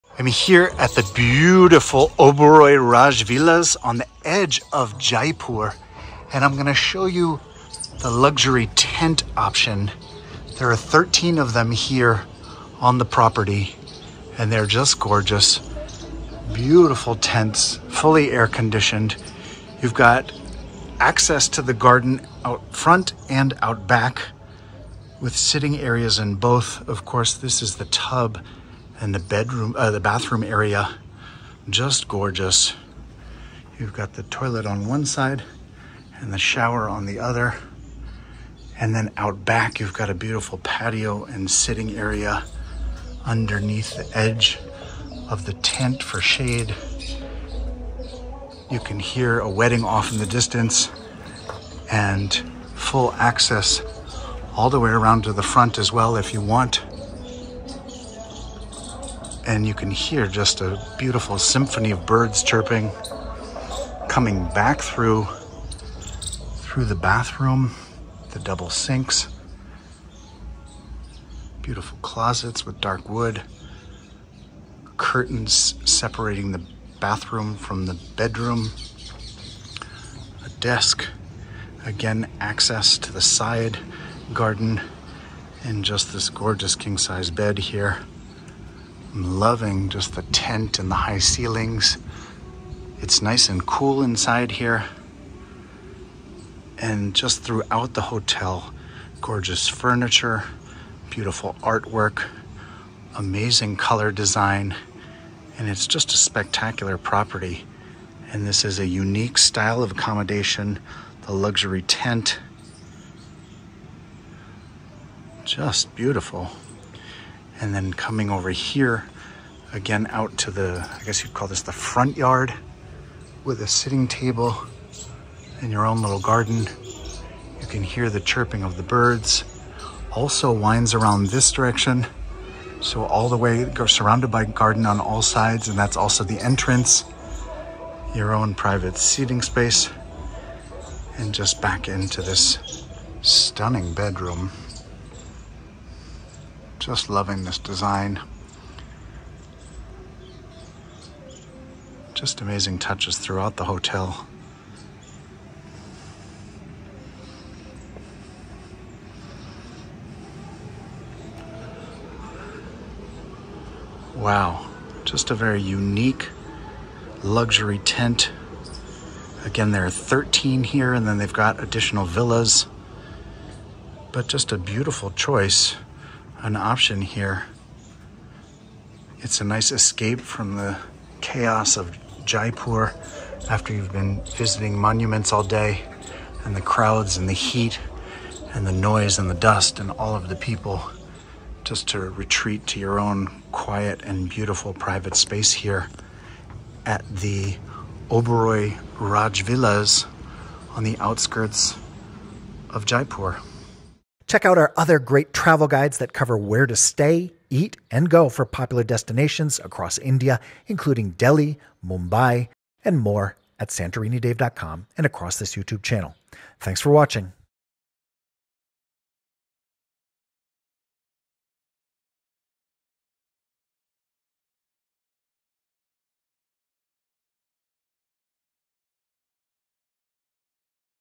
Jaipur Luxury Hotel - Oberoi Rajvilas - Tent Room Tour in Rajasthan
Jaipur's top luxury hotel The Oberoi Rajvilas offers unique 5-star accommodations. Set amid beautiful greenery, this luxurious property features a giant pool, and 3 dining options,

We've got lots more Indian luxury hotel suite and villa tours across this channel.